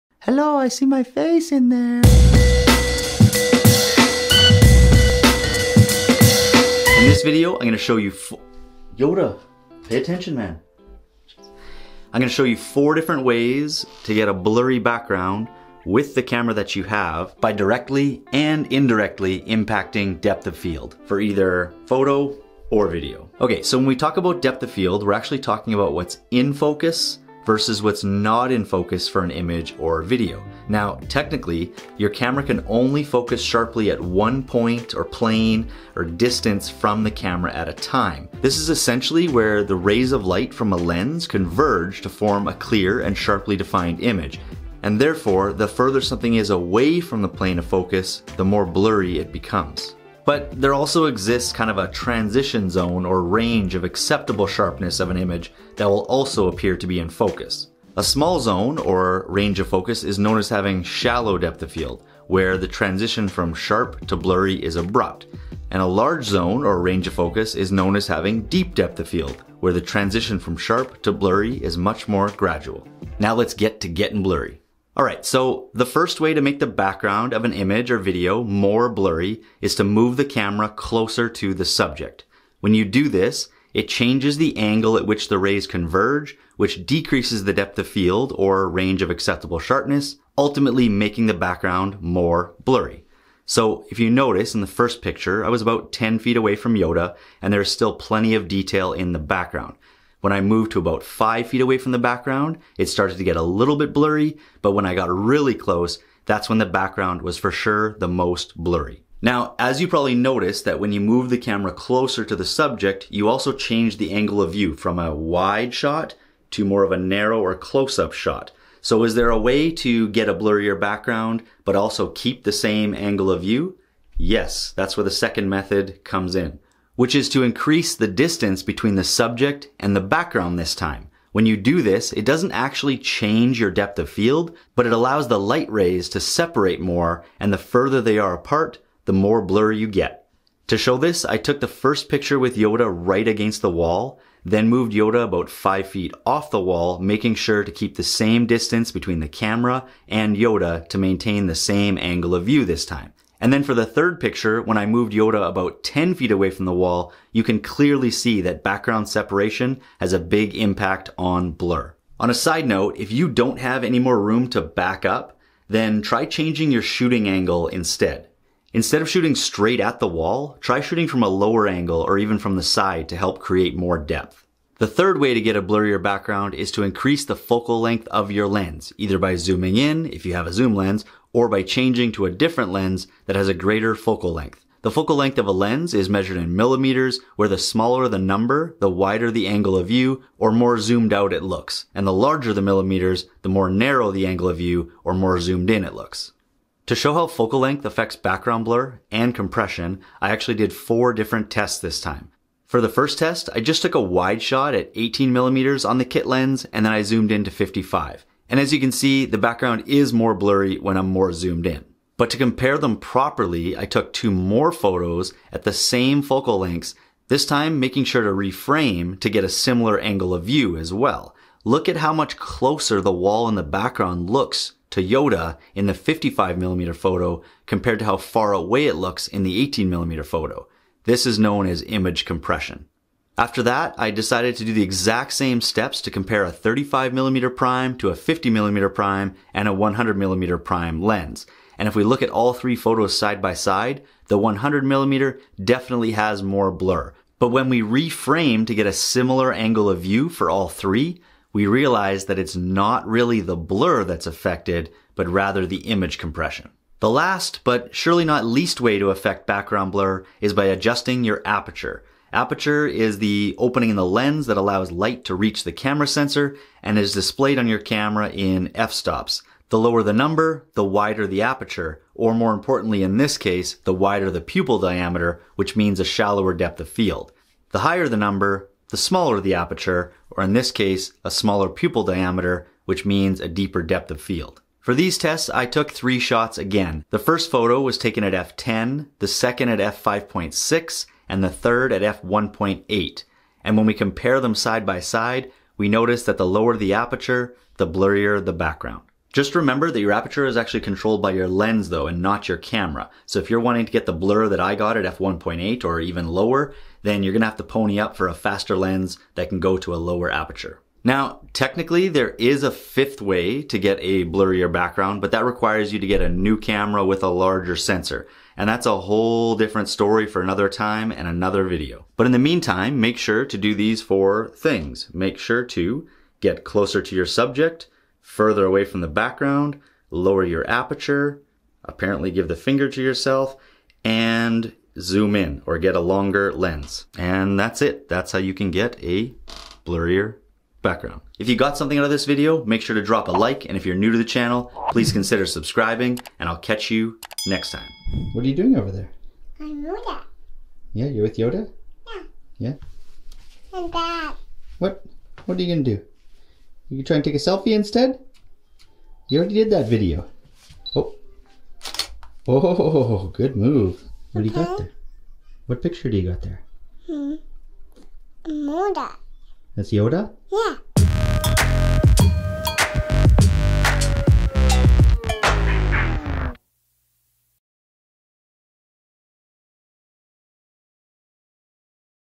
How to Blur Background in Photos and Video | DSLR Depth of Field Tutorial - Bokeh
In this camera tutorial I use the Vintage 1981 Yoda Hand Puppet to show the 4 different ways that you can get more background blur, with the camera and lens that you already have, by directly and indirectly impacting Depth of Field (shallow Depth of Field vs Deep Depth of Field) for both photography and video. I got this Vintage Yoda Puppet in 1981, when I was 1 year old!

0:00 Start
0:23 Depth of Field (overview)
1:13 Camera to Subject
1:58 Subject to Background
2:53 Focal Length of Lens
4:16 Adjust the Aperture